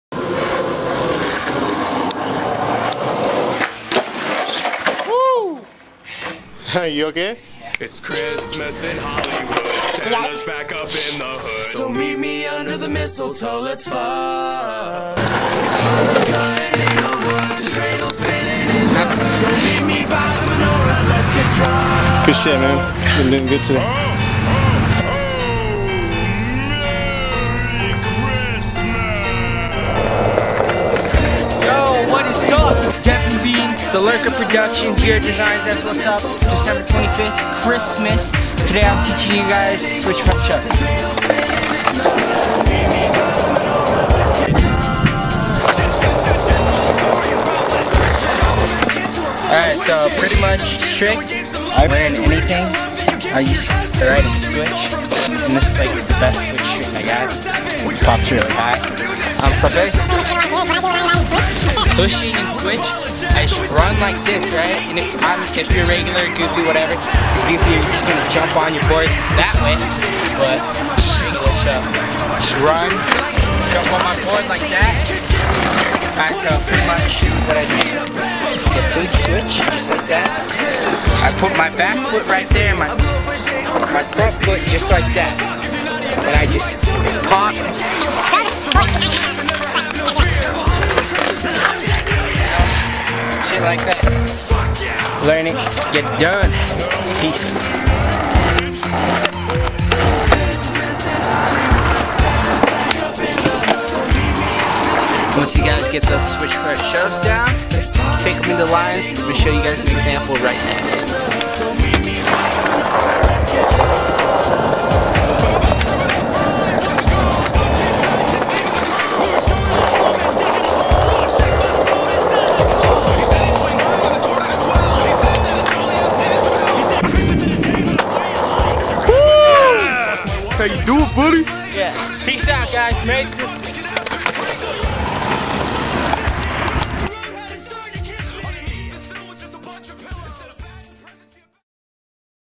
Christmas with the lurks switch front shuv trick tip
Devin Bean with the switch front shuv trick tip on Christmas peep at it fools!